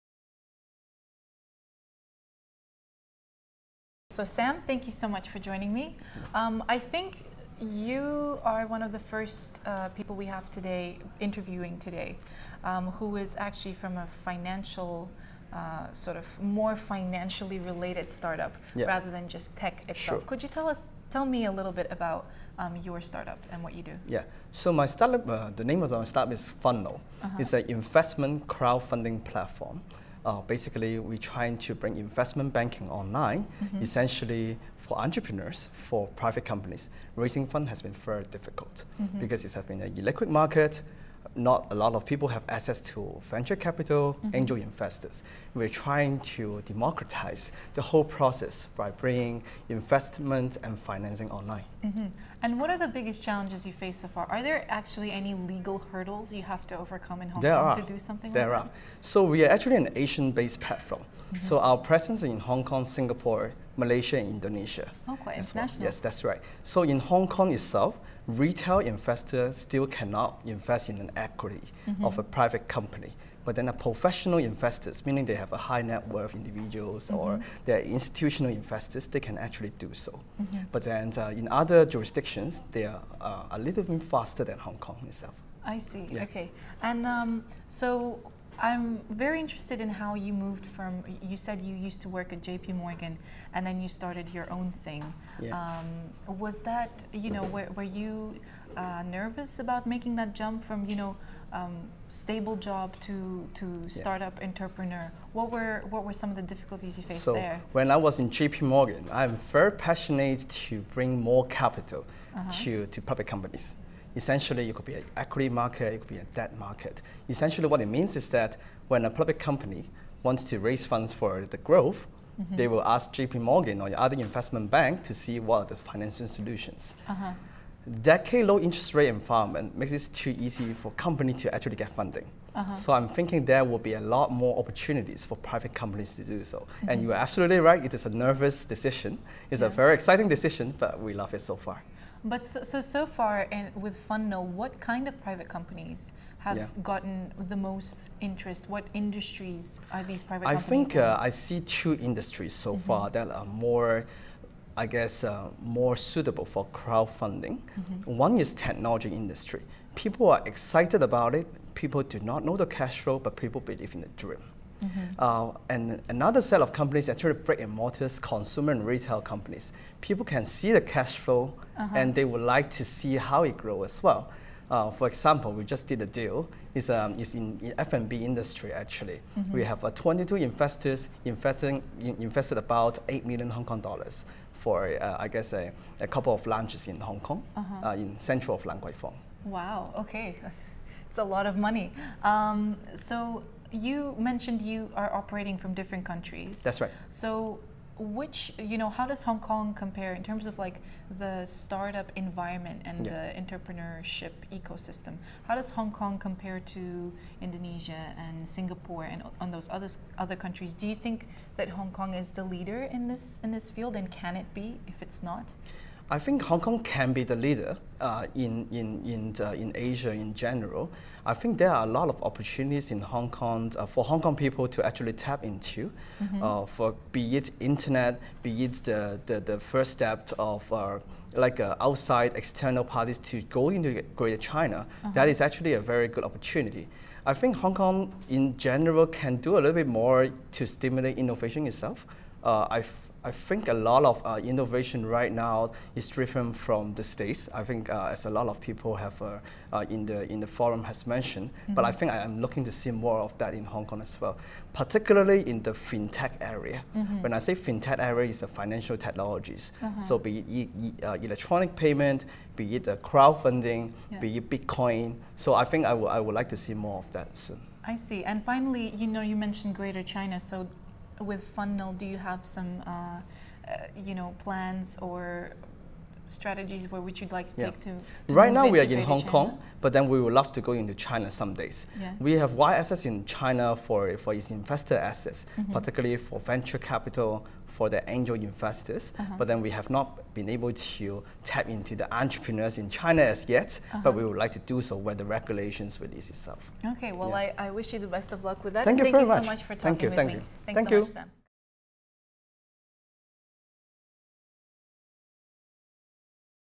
HKU DREAMCATCHERS Interview: Sam Ng
Dreamcatchers - Inaugural Forum of HKU's  Entrepreneurship Series
on May 31, 2015.

Interviewee: Sam Ng
Interviewer: Audrey Kabilova
Produced by the Journalism and Media Studies Centre, HKU